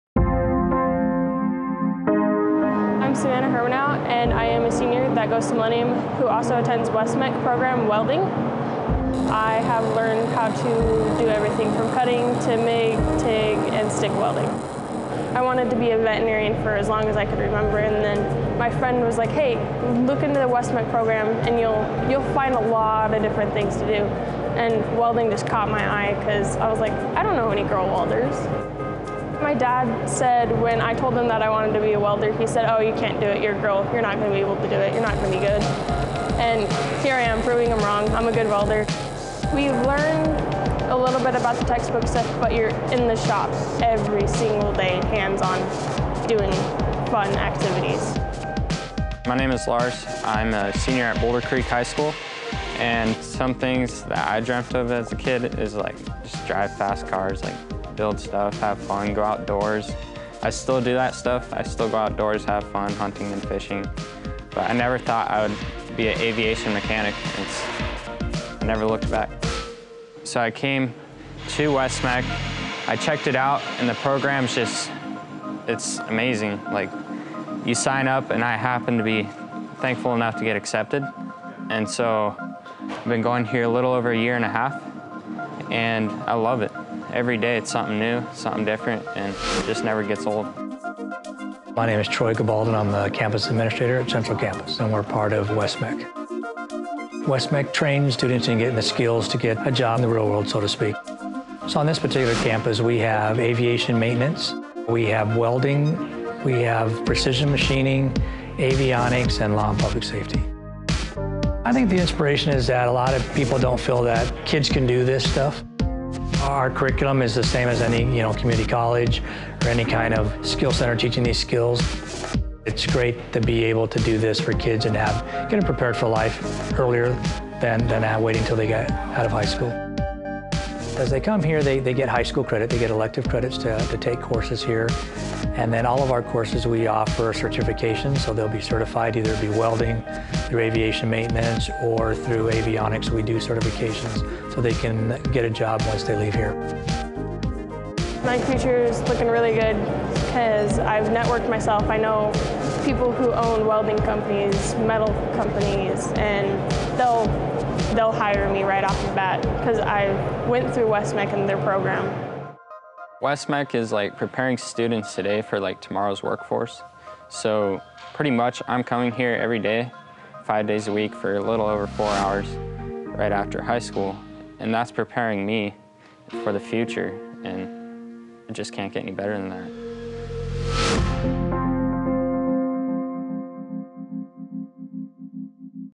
West MEC Provides an Alternative to College or a Smart Way to Pay for It
The Western Maricopa Education Center, or ‘West MEC’ is helping high school students develop marketable vocational skills, providing an alternative to college, or a smart way to pay for it.